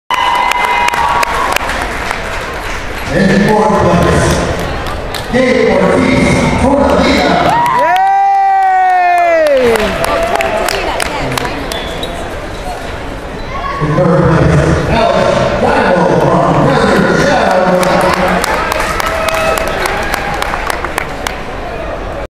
Gabe Ortiz takes 4th AZ State Wrestling Championships
Gabe 4-2 takes 4th takes out 5th,4th and 6th seed